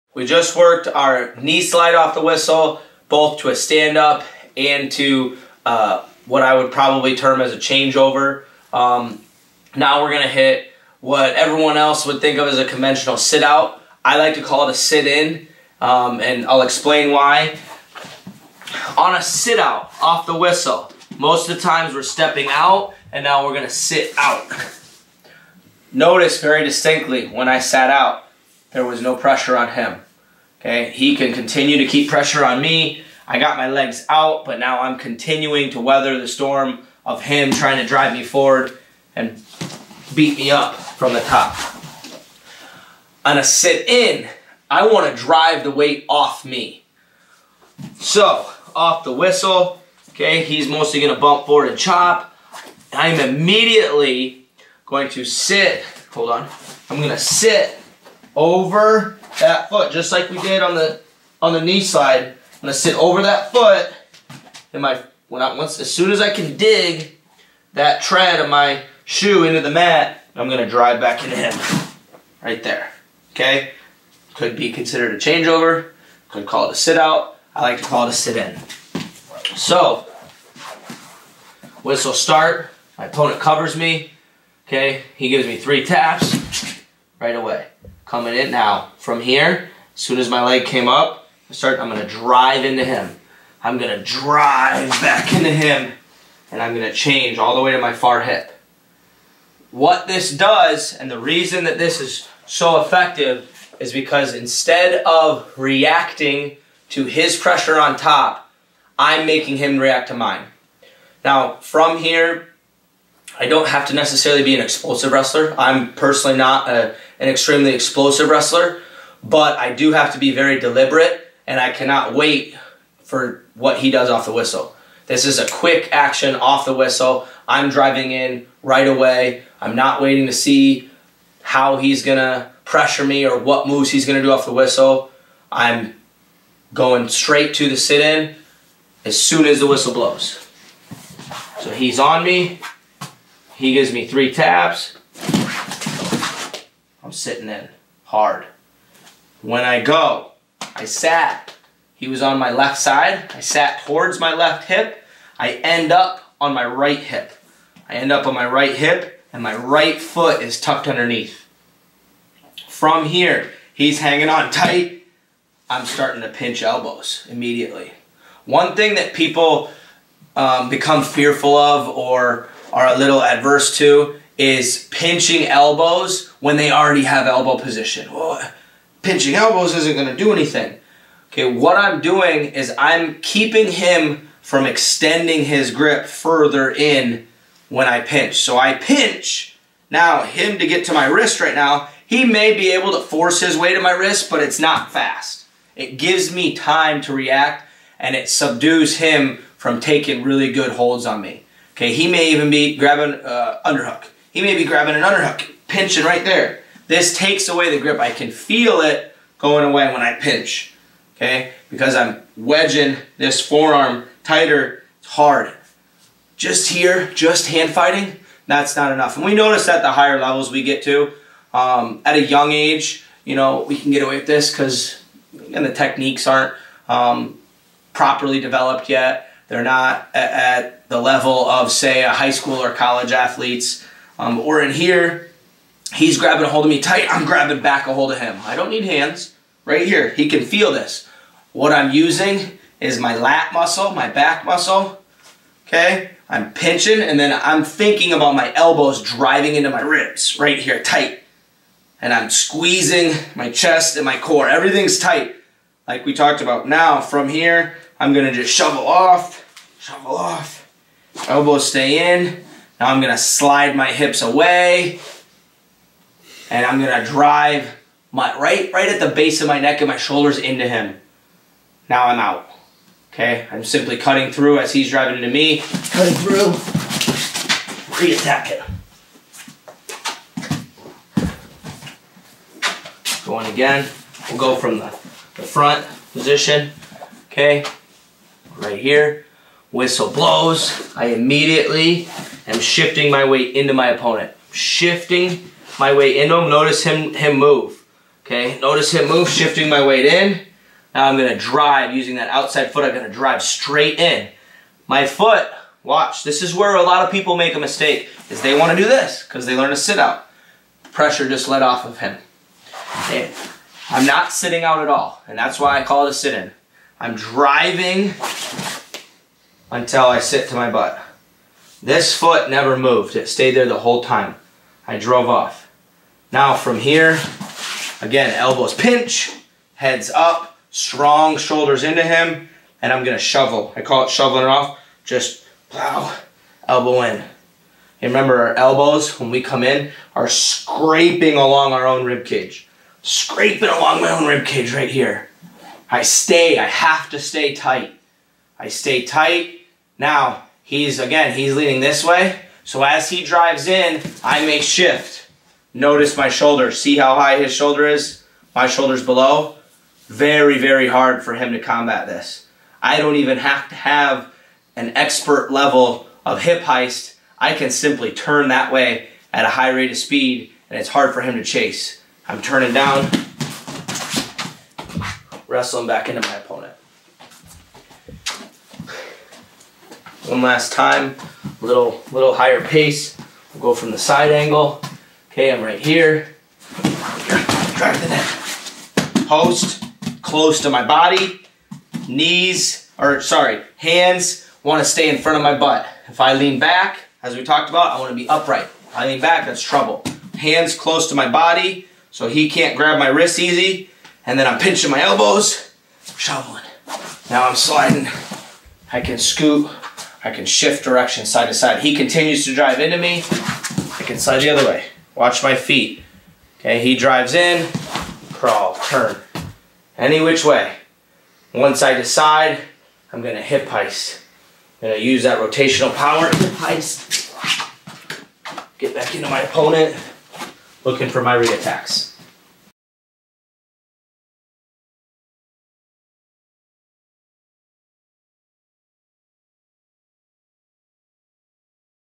Sit In Off The Whistle by Matt McDonough
Learn how to sit out off the whistle, especially to defend leg riders, with Matt McDonough